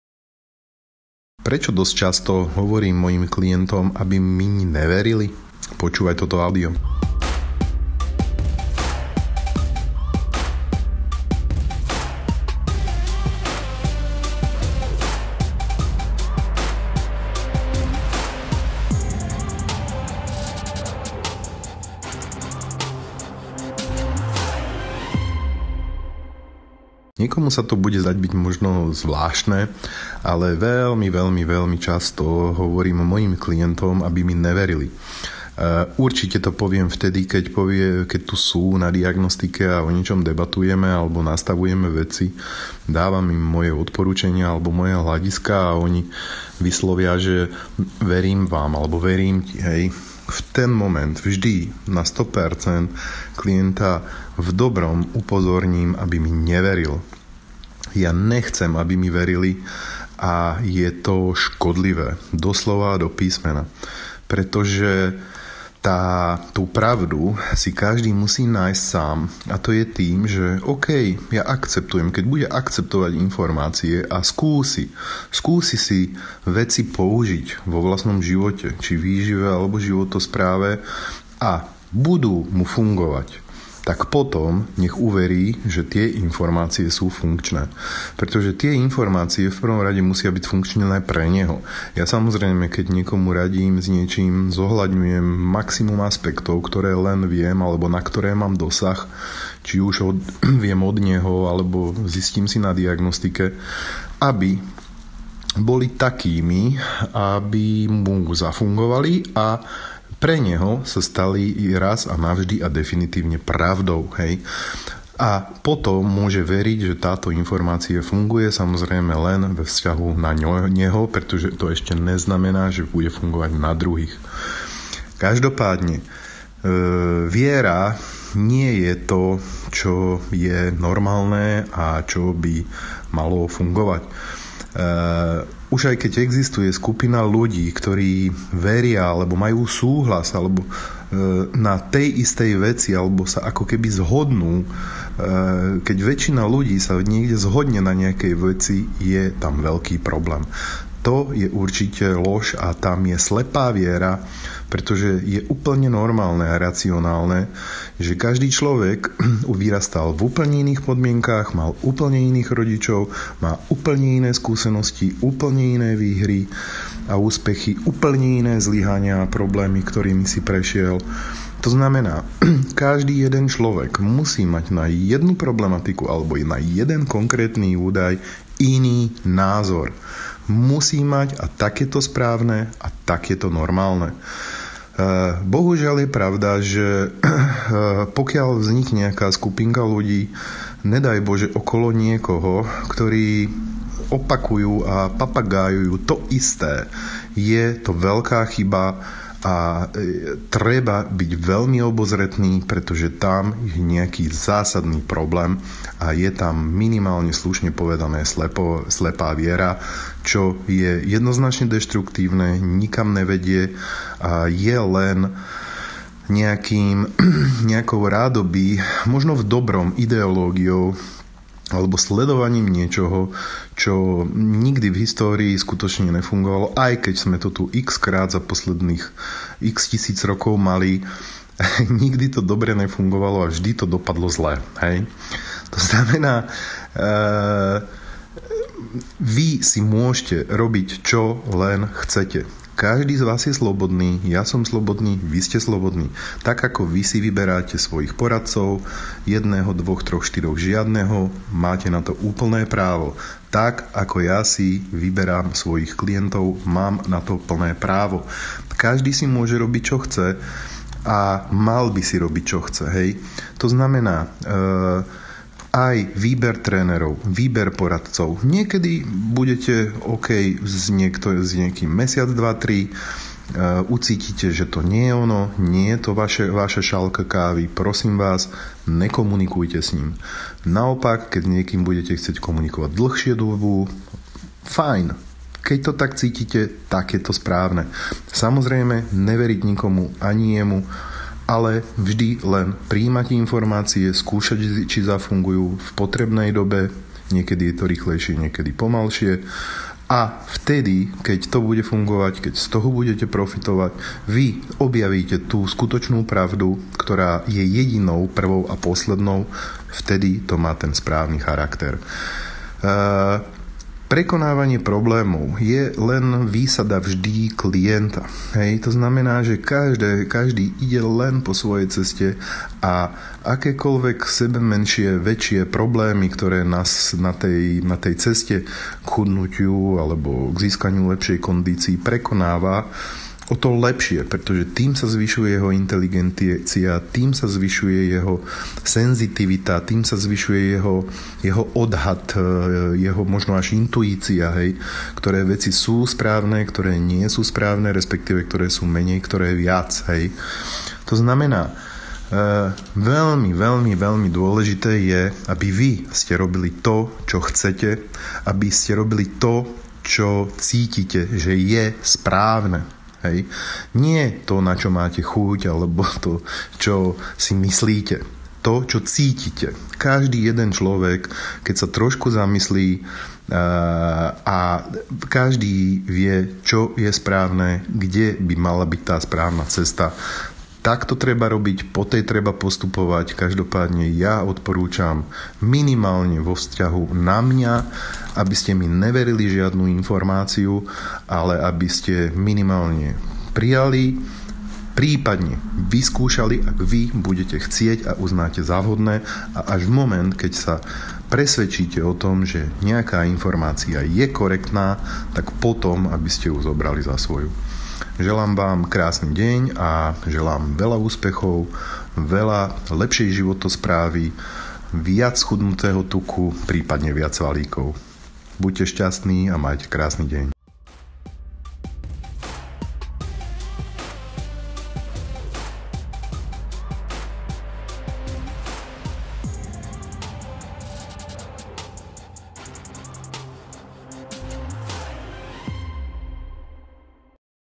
NEVERTE MI ANI SLOVO 🔴 Jankowitch © 2018
🔴 Aké potraviny sú pre teba nevhodné ?

Upozornenie: Uvedené informácie majú len informatívny charakter a sú vyjadrením osobného názoru autora. Informácie ohladom zdravia konzultujte výhradne so svojím lekárom.